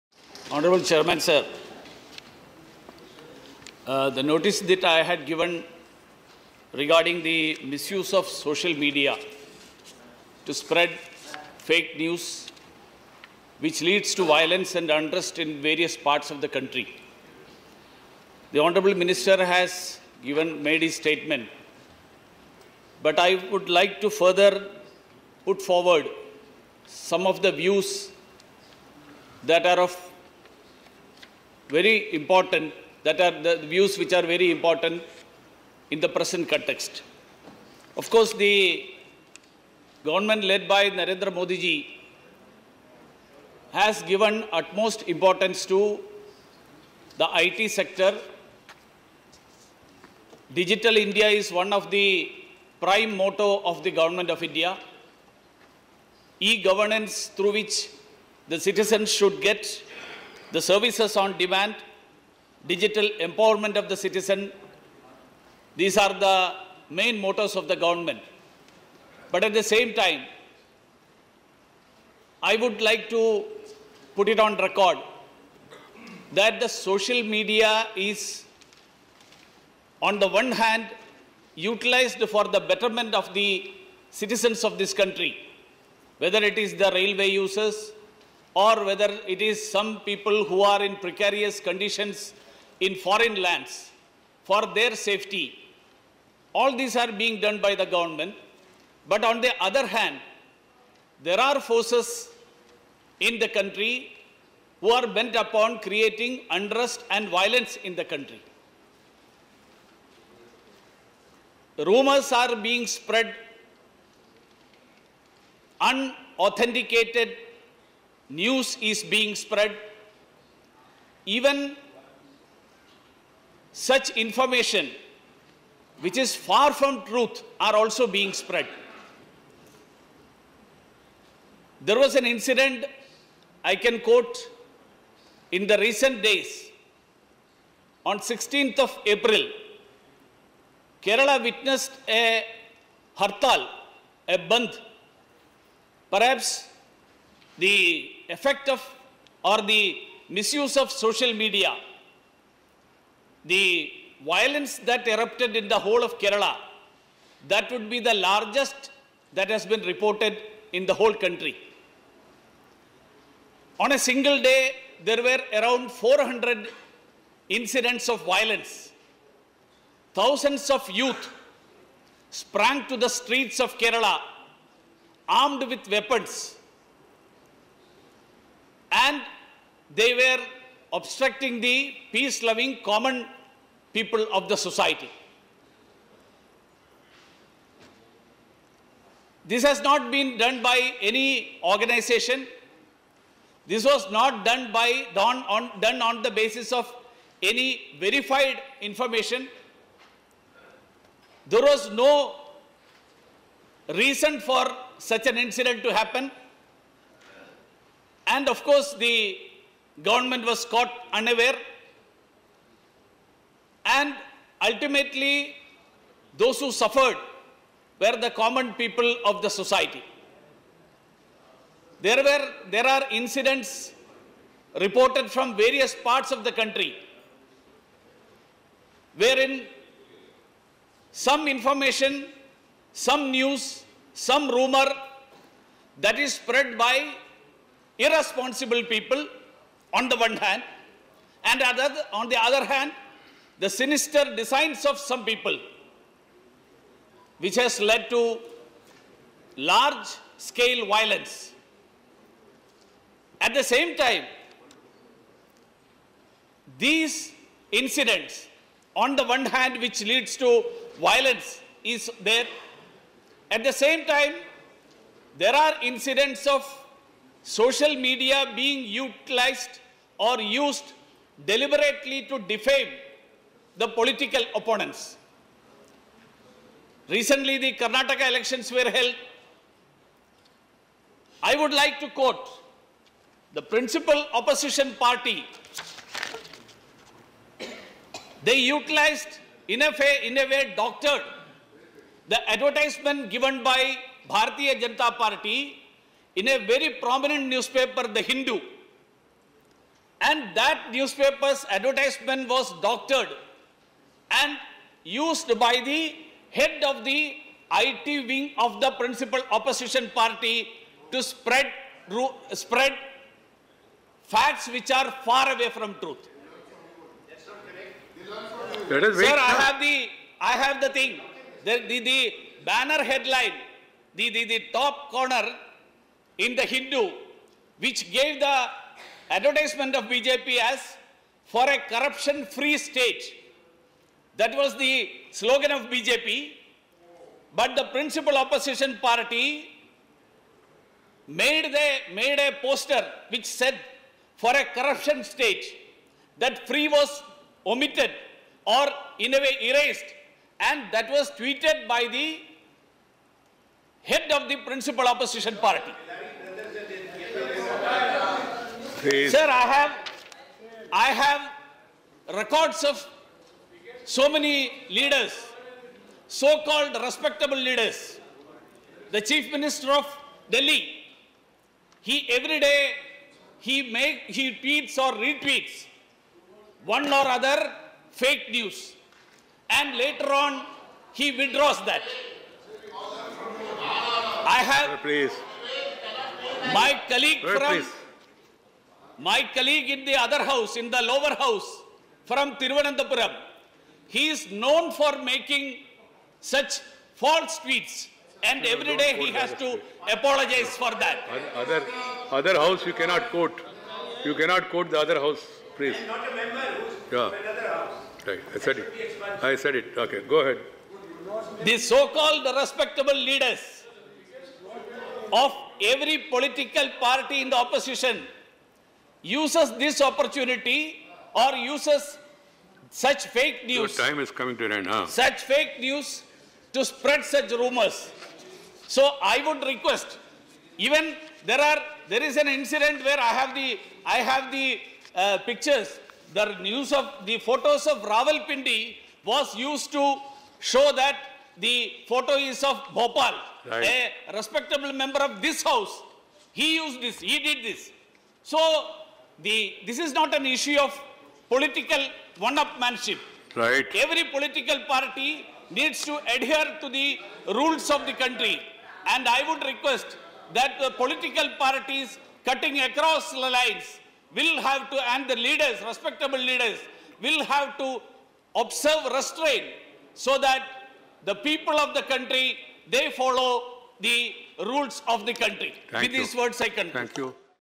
Shri V. Muraleedharan on misuse of social media platforms to spread rumours and fake news
Shri V. Muraleedharan on misuse of social media platforms to spread rumours and fake news in Rajya Sabha, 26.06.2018